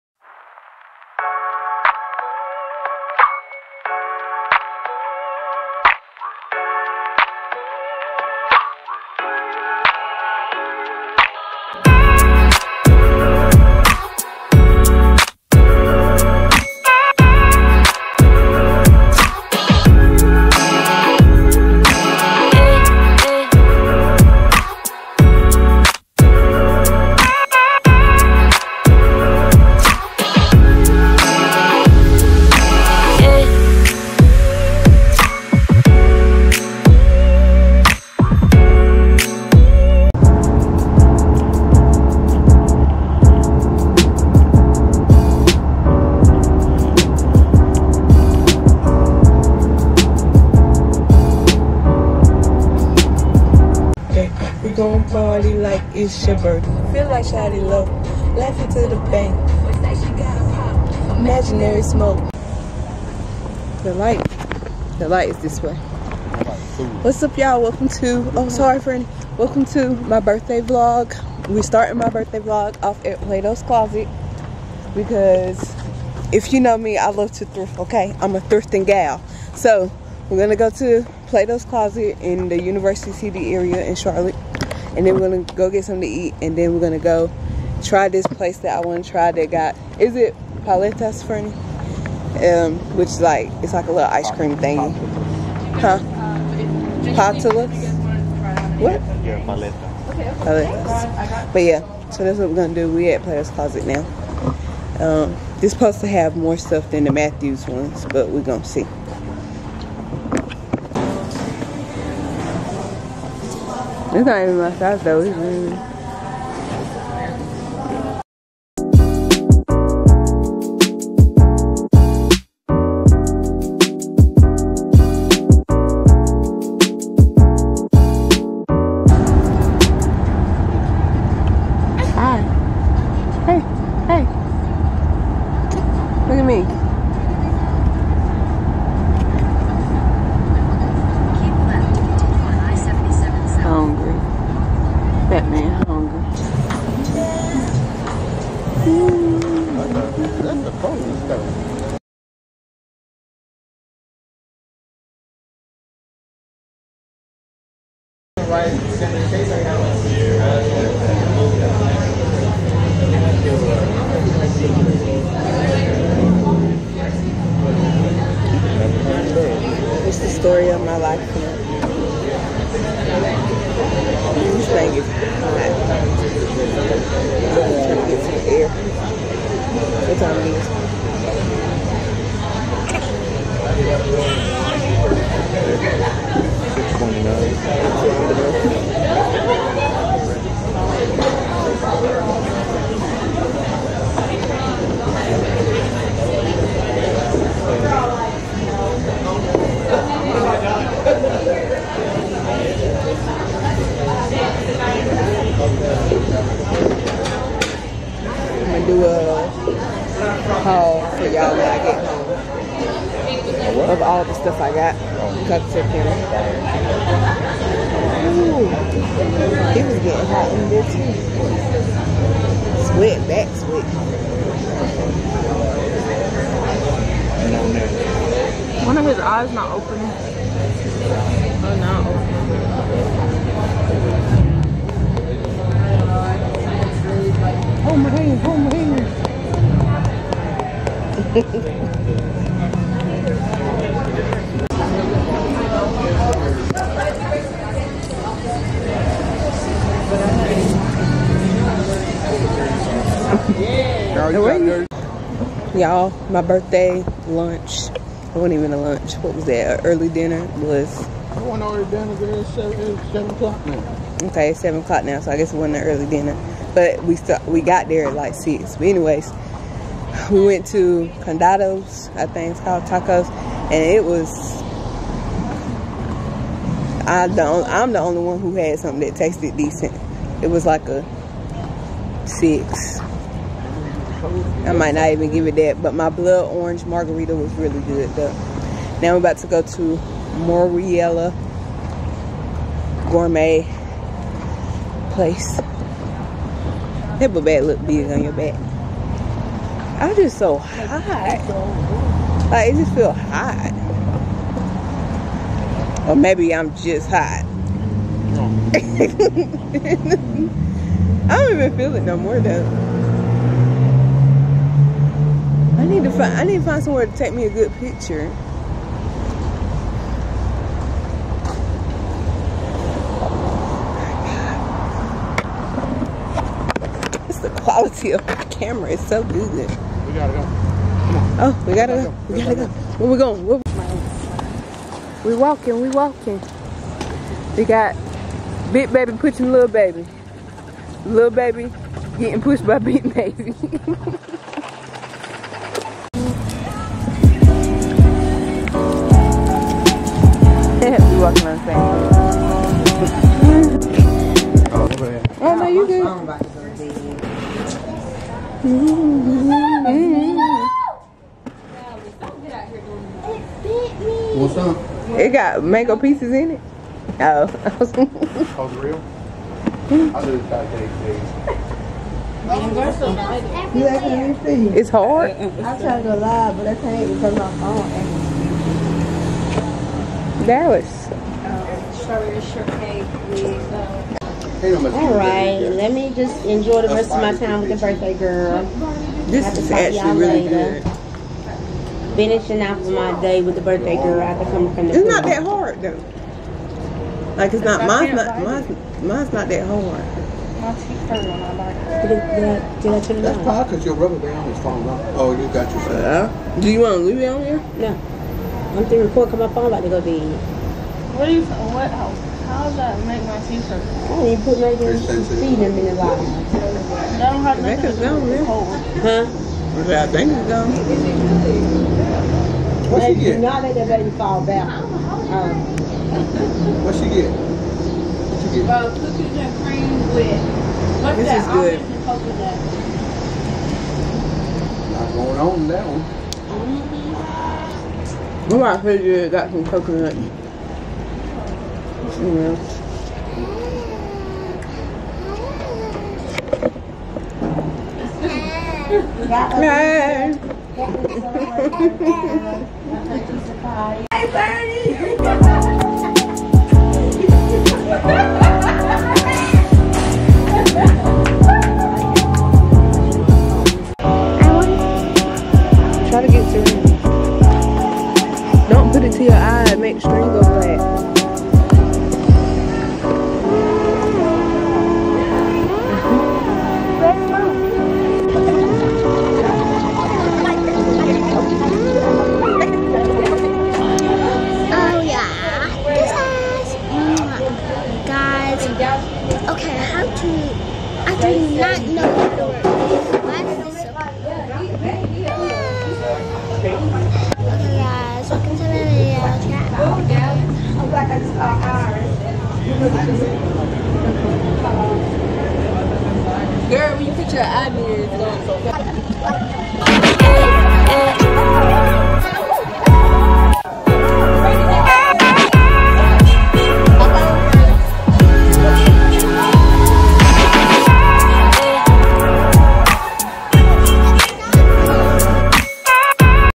Birthday Vlog: GRWM, Shopping, Dinner, Paletas Ice Cream, Exploring, and Family Time, Mini Vlog
hey yall,

Love Chiya